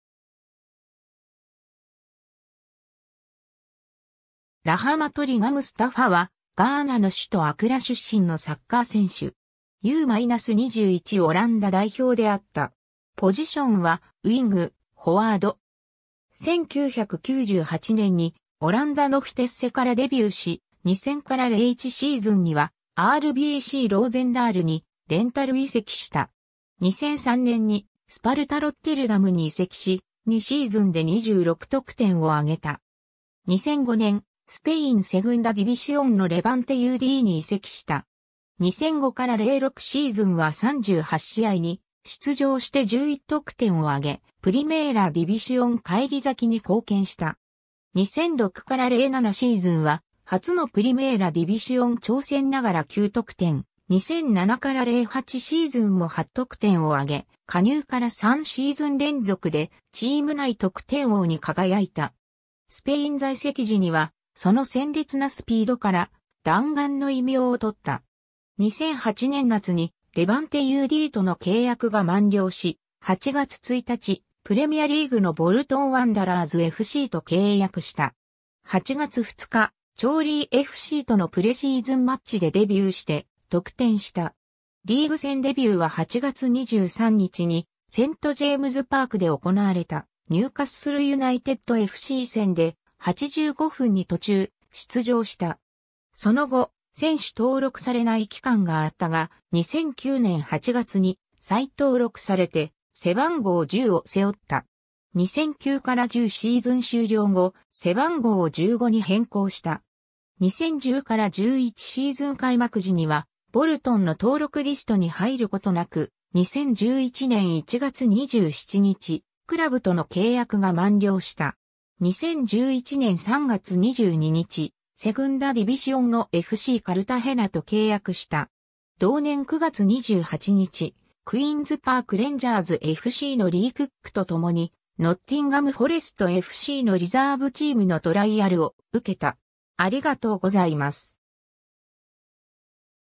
ムスタファ・リガ
スパルタ・ロッテルダムの選手
ボルトン・ワンダラーズFCの選手
ラハマト・リガ・ムスタファ（Rahamat Riga Mustapha、1981年10月10日 - ）は、ガーナの首都アクラ出身のサッカー選手。
U-21オランダ代表であった。
ポジションはウイング、フォワード。
 1998年にオランダのフィテッセからデビューし、2000-01シーズンにはRBCローゼンダールにレンタル移籍した。
2003年にスパルタ・ロッテルダムに移籍し、2シーズンで26得点を挙げた。
 2005年、スペイン・セグンダ・ディビシオン（2部）のレバンテUDに移籍した。
2005-06シーズンは38試合に出場して11得点を挙げ、プリメーラ・ディビシオン（1部）返り咲きに貢献した。
2006-07シーズンは初のプリメーラ・ディビシオン挑戦ながら9得点、2007-08シーズンも8得点を挙げ、加入から3シーズン連続でチーム内得点王に輝いた。
スペイン在籍時には、その鮮烈なスピードから弾丸(The Bullet)の異名を取った。
 2008年夏にレバンテUDとの契約が満了し、8月1日、プレミアリーグのボルトン・ワンダラーズFCと契約した。
8月2日、チョーリーFCとのプレシーズンマッチ(2-0)でデビューして得点した。
リーグ戦デビューは8月23日にセント・ジェームズ・パークで行われたニューカッスル・ユナイテッドFC戦で、85分に途中出場した。
その後、選手登録されない期間があったが、2009年8月に再登録されて背番号10を背負った。
2009-10シーズン終了後、背番号を15に変更した。
2010-11シーズン開幕時にはボルトンの登録リストに入ることなく、2011年1月27日、クラブとの契約が満了した。
 2011年3月22日、セグンダ・ディビシオン（2部）のFCカルタヘナと契約した。
同年9月28日、クイーンズ・パーク・レンジャーズFCのリー・クックとともにノッティンガム・フォレストFCのリザーブチームのトライアルを受けた。